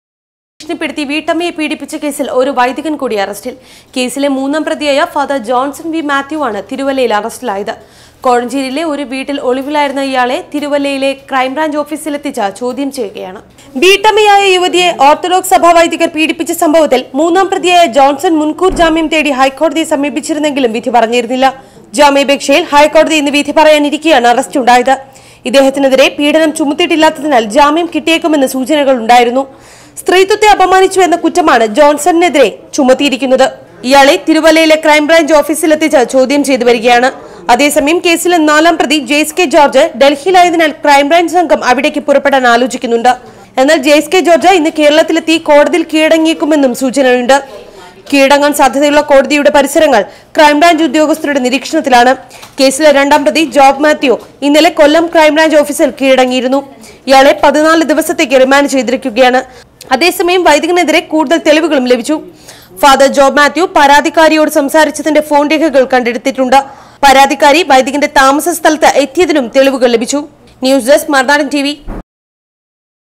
വീട്ടമ്മയെ പീഡിപ്പിച്ച കേസില്‍ ഒരു വൈദികന്‍ കൂടി അറസ്റ്റില്‍ | Orthodox Church case
വീട്ടമ്മയെ പീഡിപ്പിച്ച കേസില്‍ ഒരു വൈദികന്‍ കൂടി അറസ്റ്റില്‍.